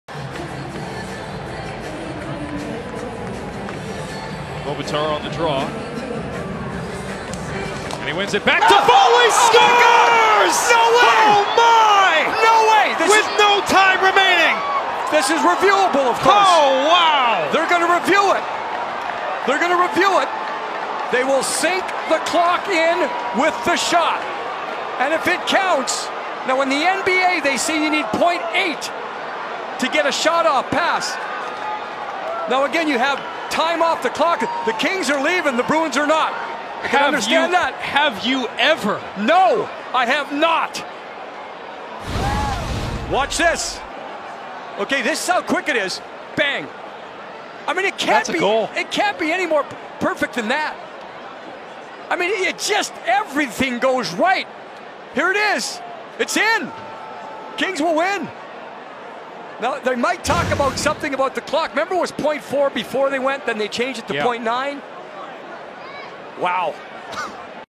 KINGS SCORE OT WINNER WITH 0.1 SECONDS LEFT!!!
WHAT!!!